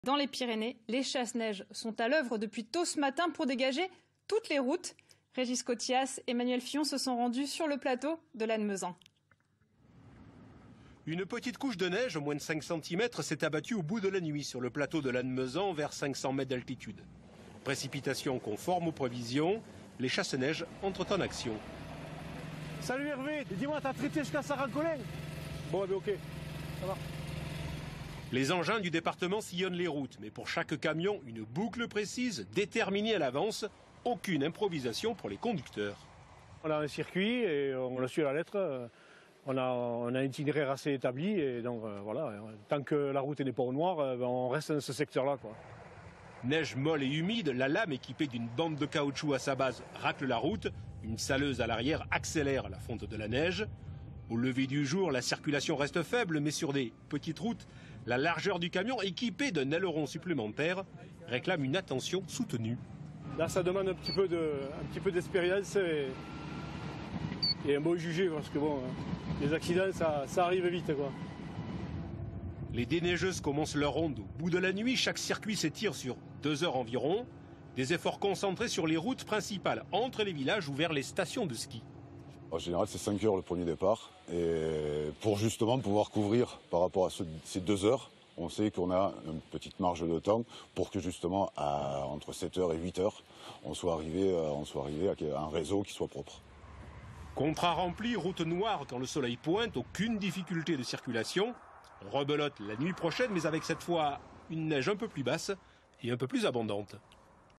Les déneigeuses des Hautes-Pyrénées à l'honneur sur France 3 Occitanie
France 3 Occitanie est venu rencontrer les agents du Département en charge de la viabilité hivernale.